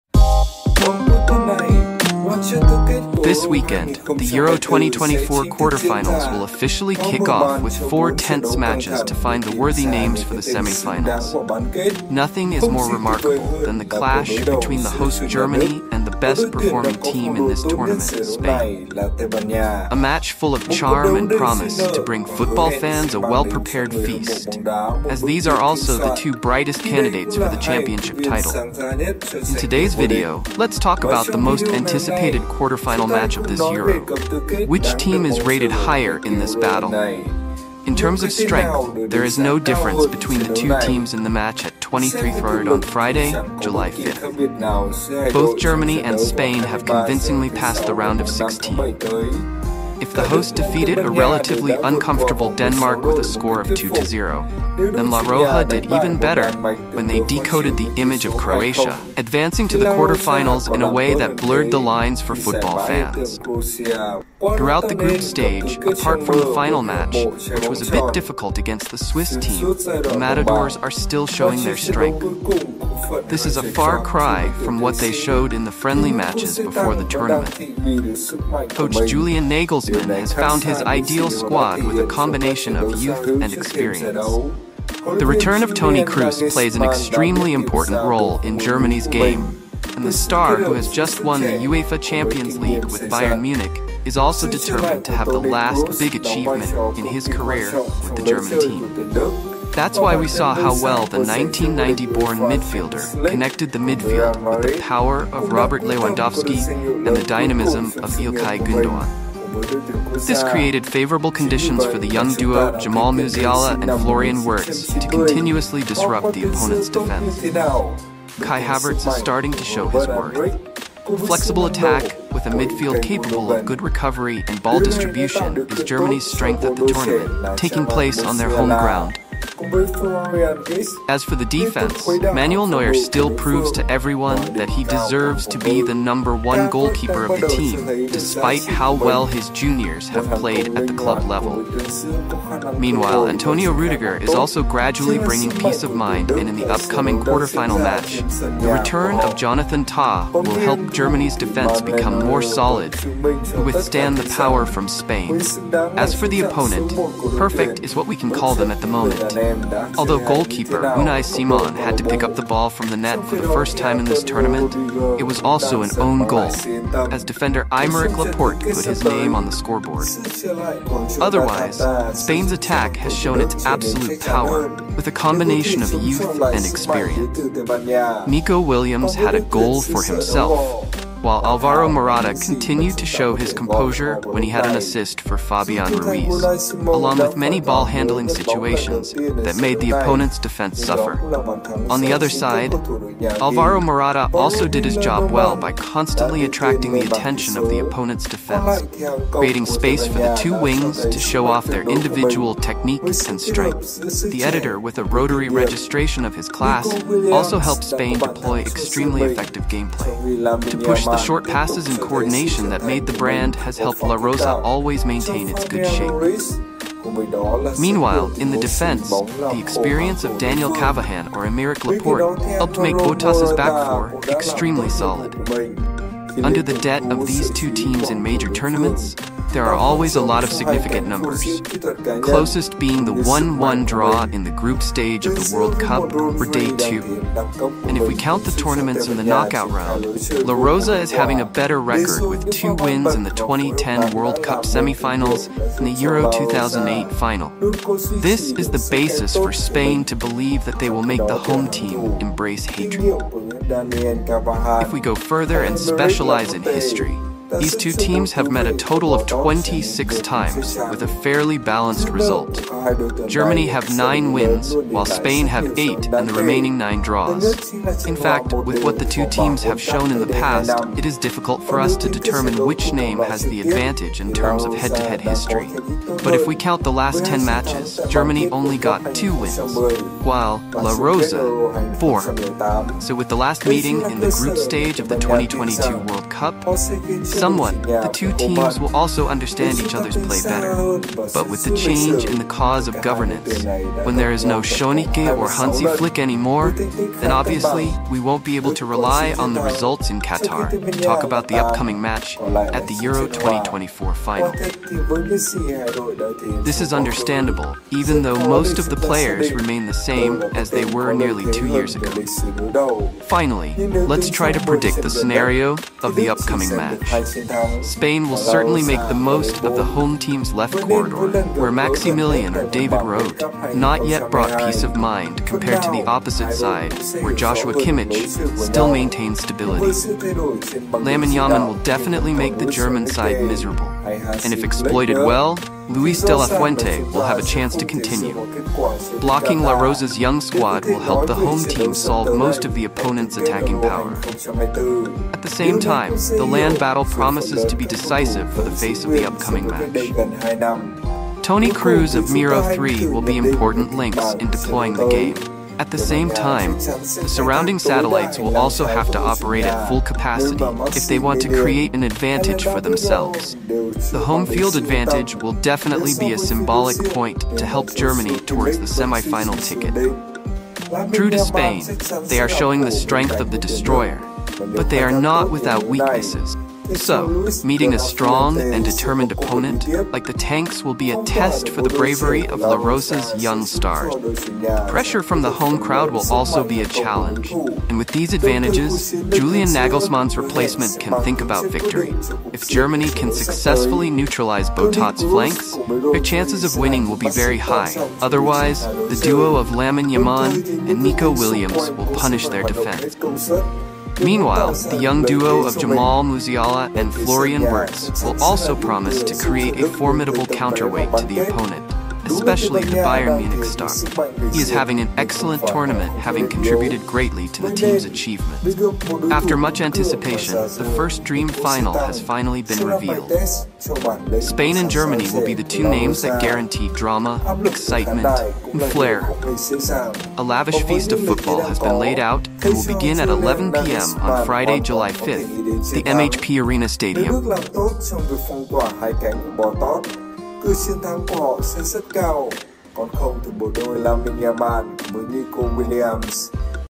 SPAIN – GERMANY IN EURO 2024 QUARTERFINALS | BATTLE OF CHAMPIONS
The highly anticipated quarterfinal clash between Germany and Spain, the tournament's top contenders, promises an epic battle for a spot in the semifinals of Euro 2024.